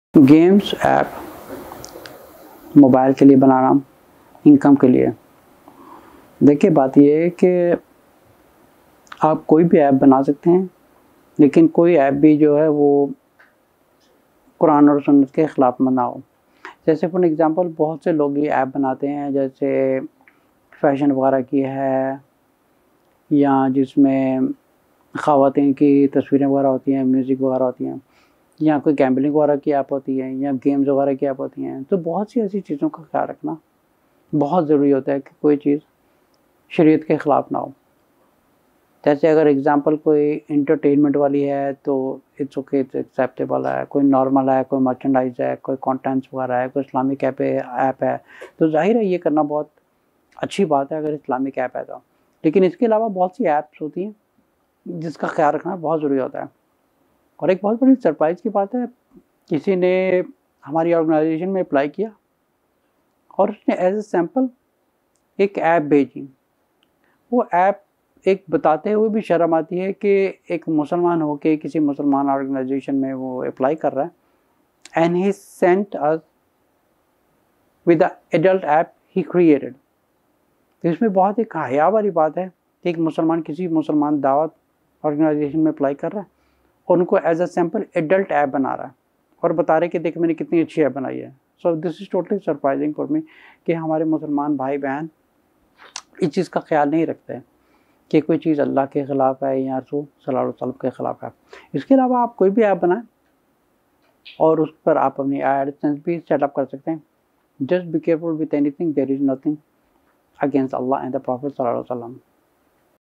Android Apple Mobile Game App Banana Jaiz Hai Income AdSense Google Ads Income Haram Dr Ammaar Saeed
ALIM University

JazakumAllah Khair